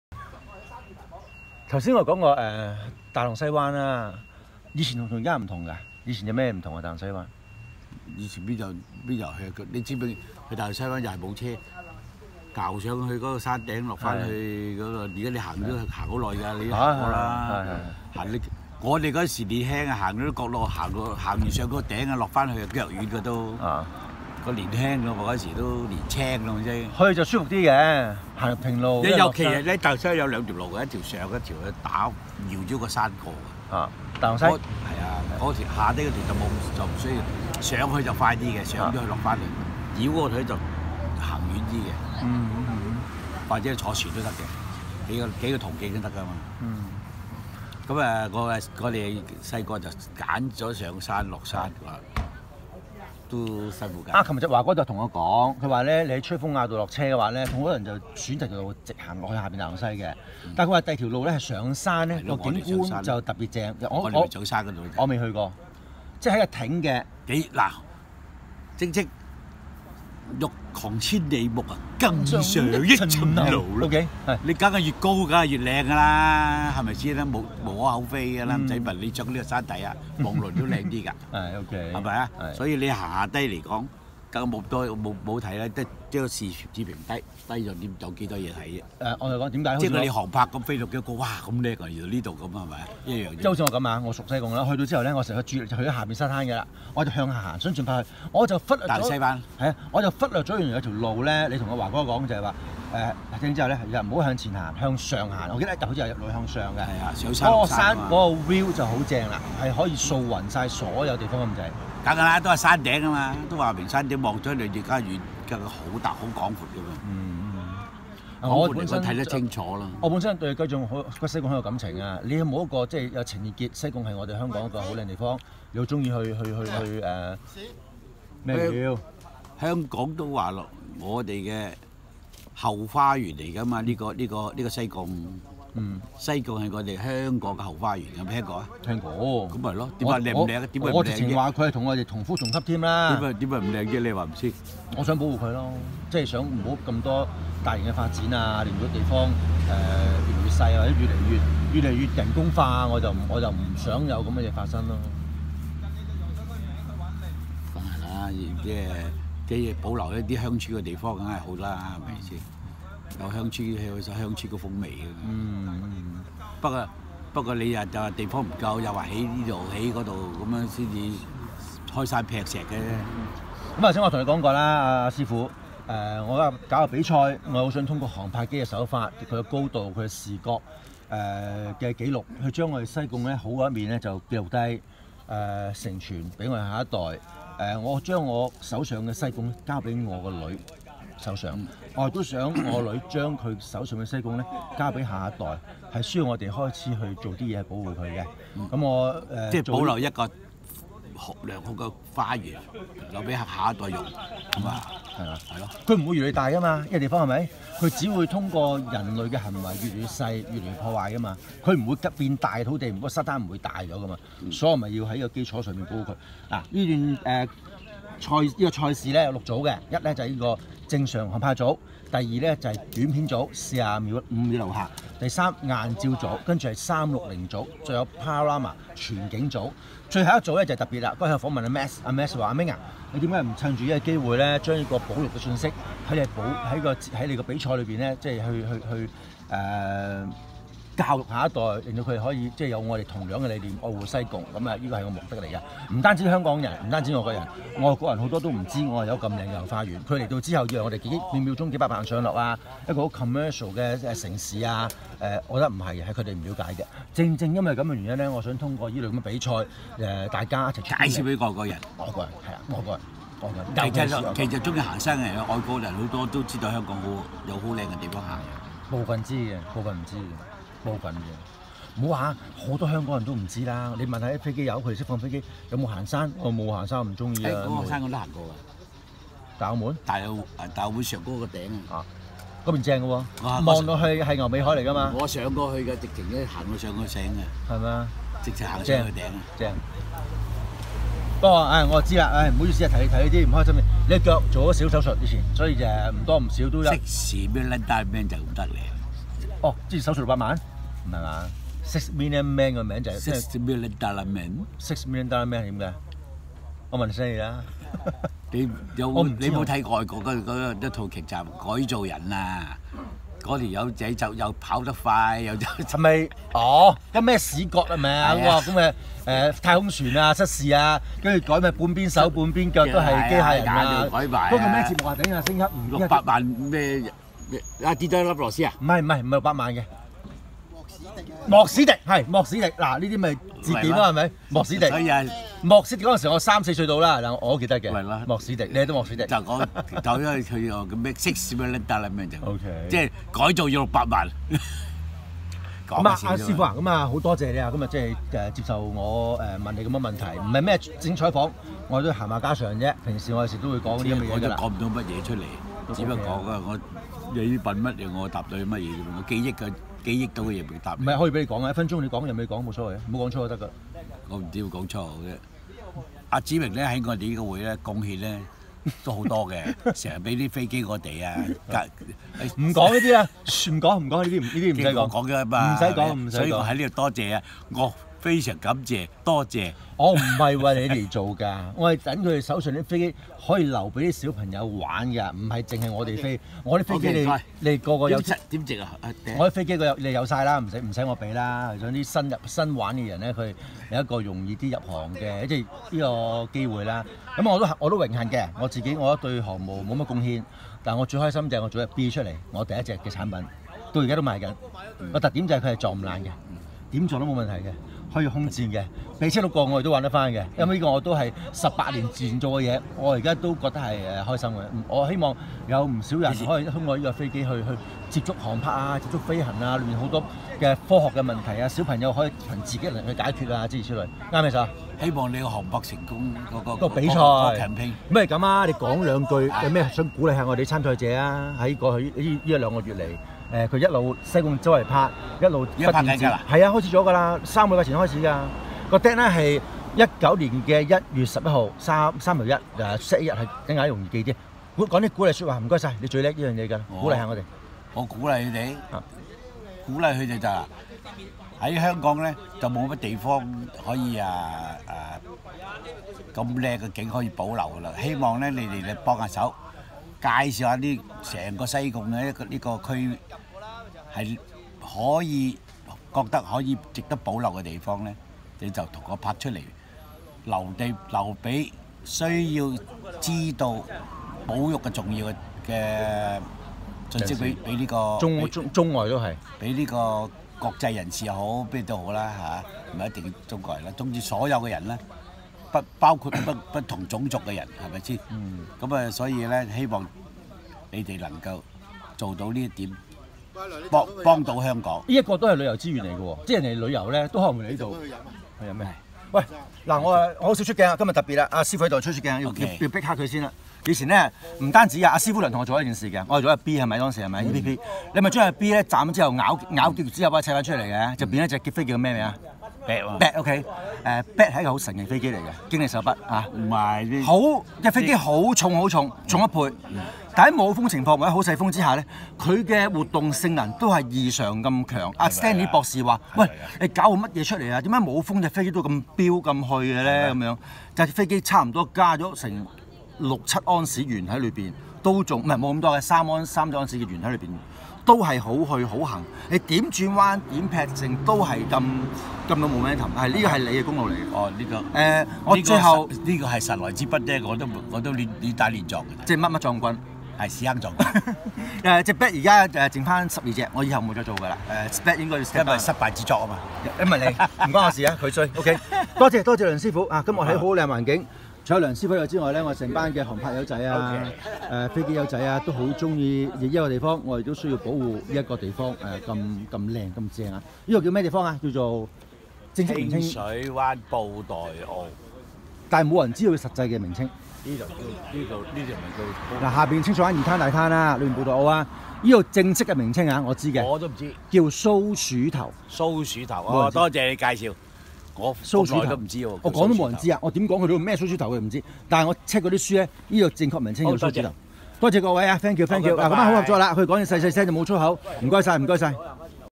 HKRCSS的創會會長良師父受訪
愛在深秋航拍比賽2018（西貢站）
前十五分鐘片段不翼而飛，否則更精彩。識了這麼多年剛剛才知道他是西貢黃麖地原居民，小時候必回老家放暑假。黃麖地in between 北潭涌和大網仔之間，以前車路只道達大網仔，再之後是北潭涌，之後便只有人行的小路。